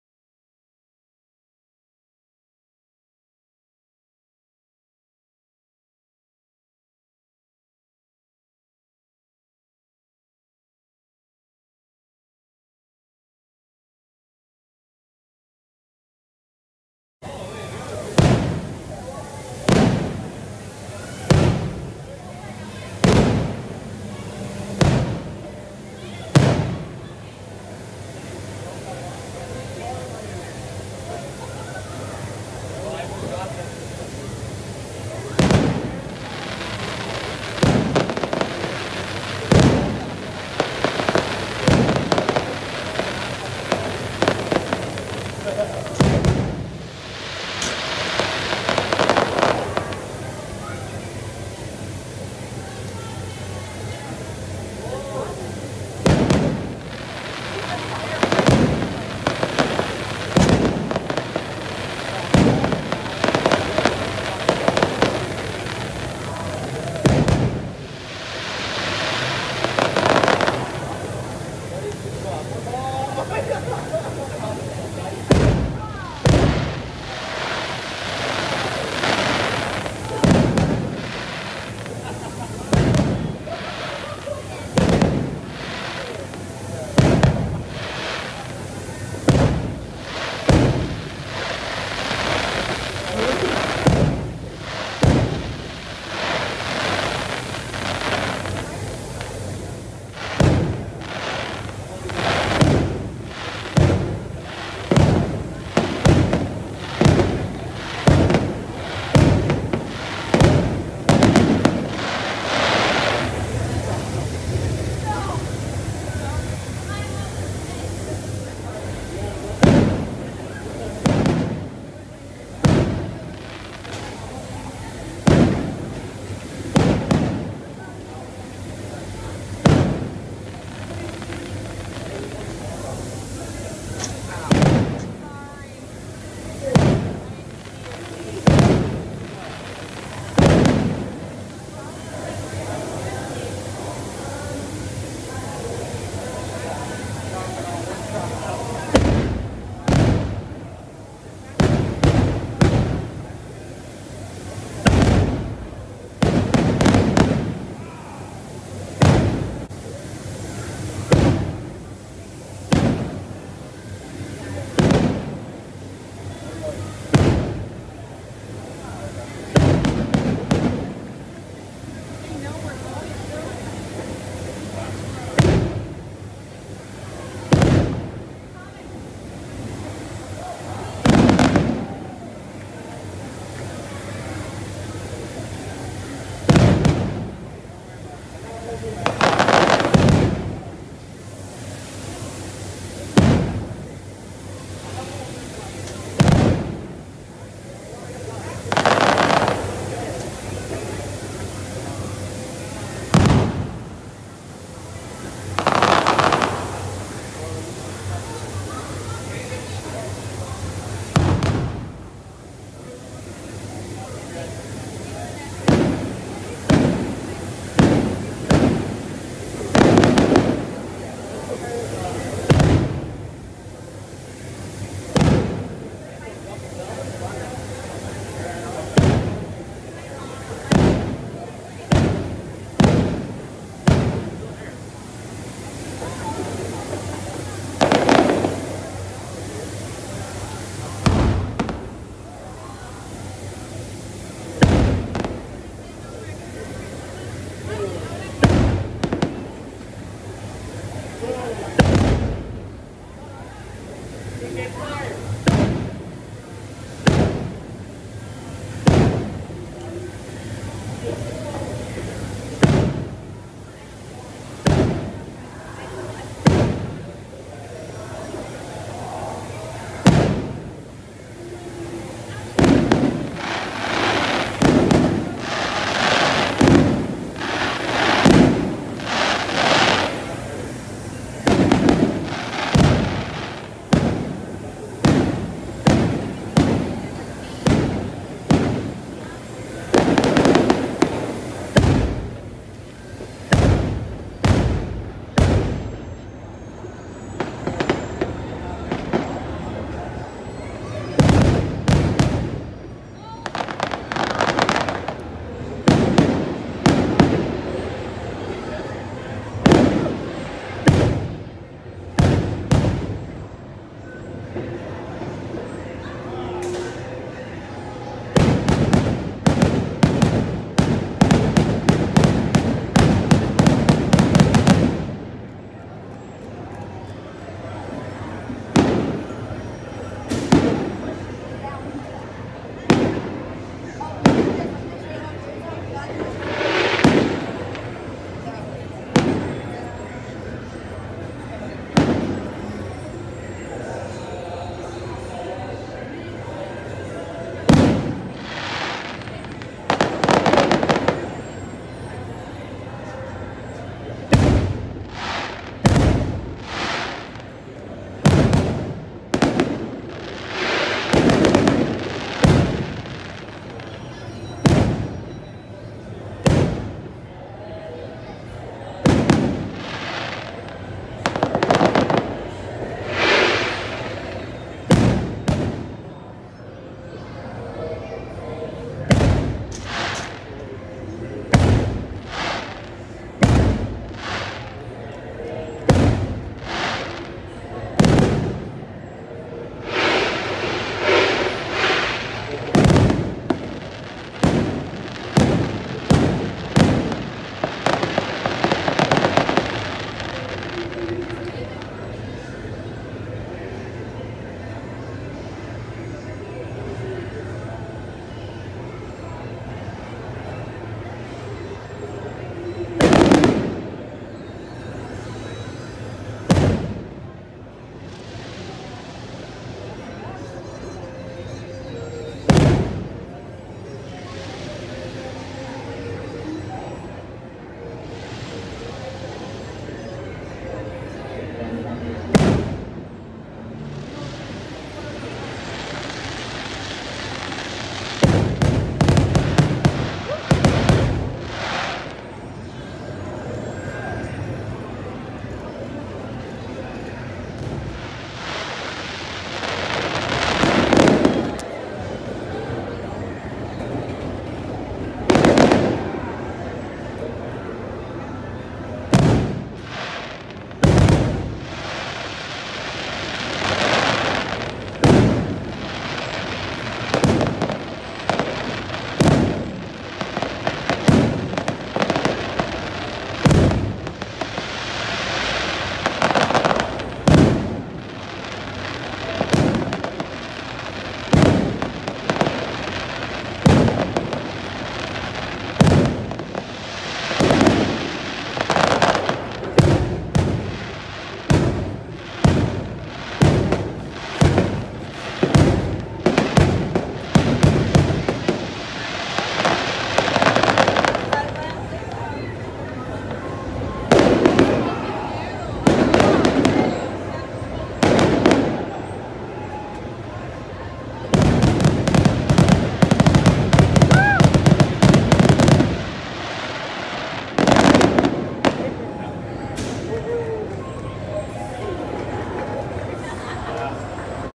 Disneyland’s Halloween Screams Fireworks 2021: Full Show Over Galaxy’s Edge (No Music)
Hi everyone! This video was taken of the full Halloween Screams fireworks show at Disneyland in California.

I took this from Star Wars: Galaxy’s Edge. This is the only location at Disneyland where the music/ soundtrack for the fireworks is not heard, as the land is supposed to represent being on a different planet.

*The fireworks are very loud in this video as a result*